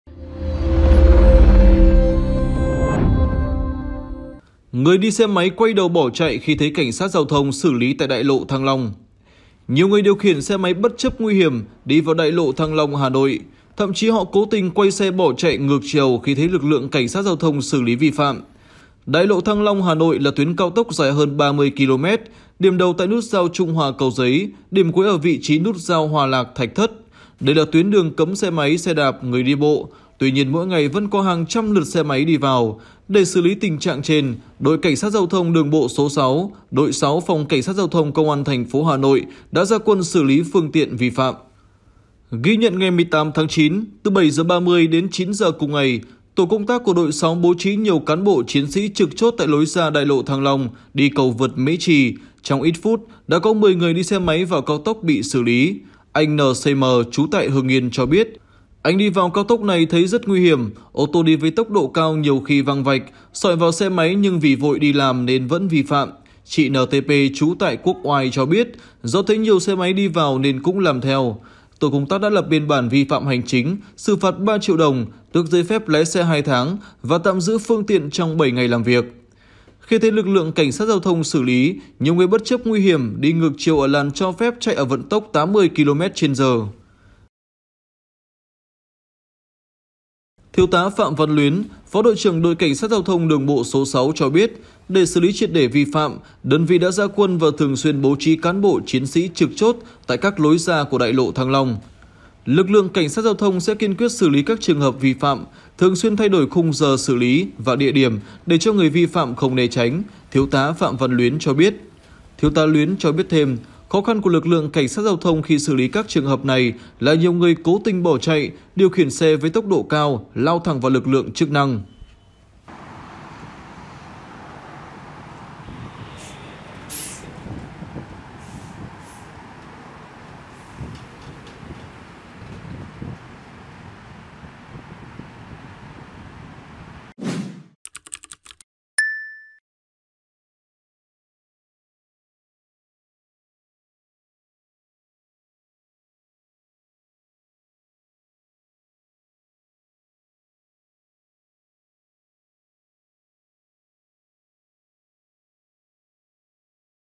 Tin tức thời sự 18/9: Người đi xe máy quay đầu bỏ chạy khi thấy CSGT xử lý tại Đại lộ Thăng Long
Đại lộ Thăng Long (Hà Nội) là tuyến cao tốc dài hơn 30km, điểm đầu tại nút giao Trung Hòa (Cầu Giấy), điểm cuối ở vị trí nút giao Hòa Lạc (Thạch Thất).
Đây là tuyến đường cấm xe máy, xe đạp, người đi bộ, tuy nhiên mỗi ngày vẫn có hàng trăm lượt xe máy đi vào.
Nhiều người điều khiển xe máy bất chấp nguy hiểm đi vào Đại lộ Thăng Long (Hà Nội). Thậm chí, họ cố tình quay xe bỏ chạy ngược chiều khi thấy lực lượng CSGT xử lý vi phạm.